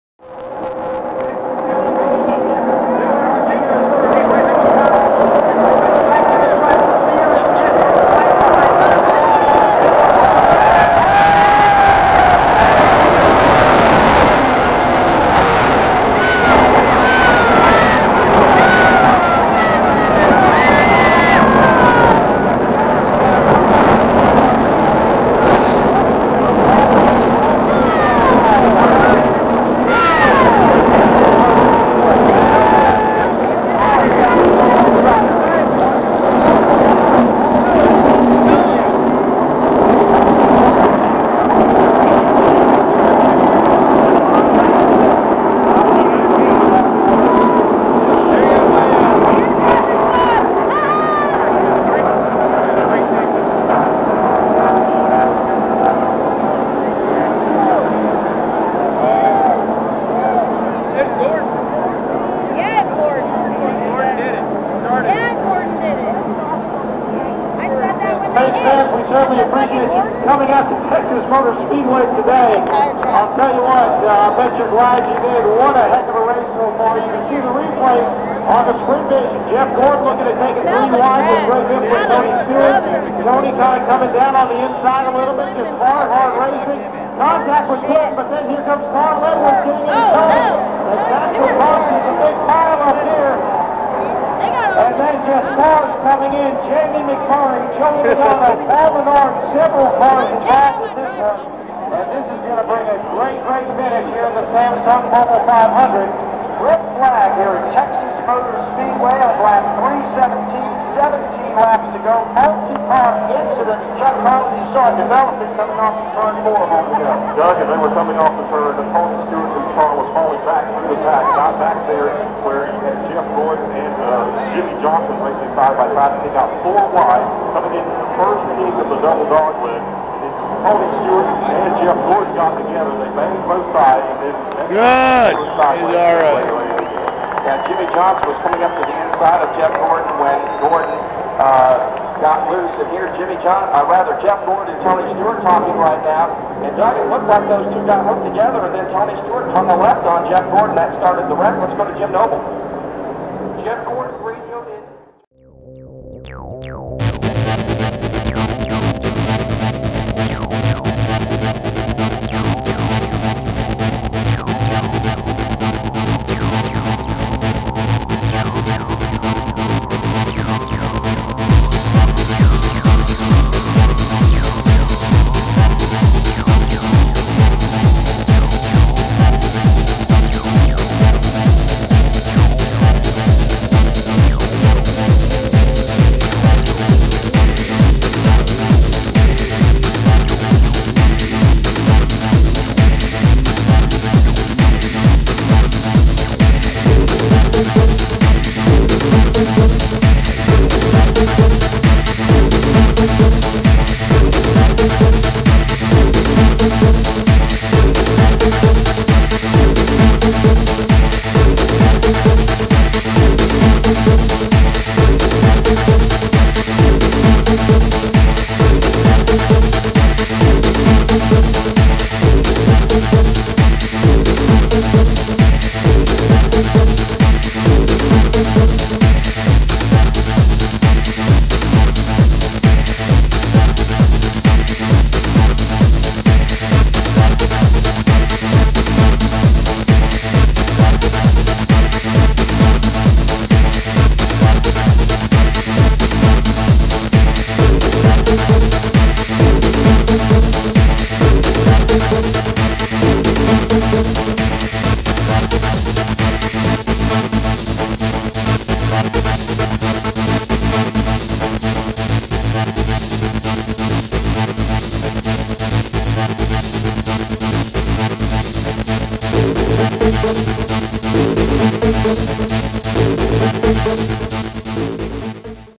Hillbilly Hellraisers invade Texas Motor speedway Wreck
on april the 18, 2010 the hillbilly hellraisers headed to the nascar track in texas. texas motor speedway. the redneck mecca as the hellraisers like to  call it.
the samsung mobile race. it was to be epic. sadly due to rain the race was canceled and rescheduled for monday april 19, 2010. this is a clip from that day. this wreck red flagged the race. it has been slowed down and then it has been way the hell slowed down for you to catch the cars and the metal colliding on track.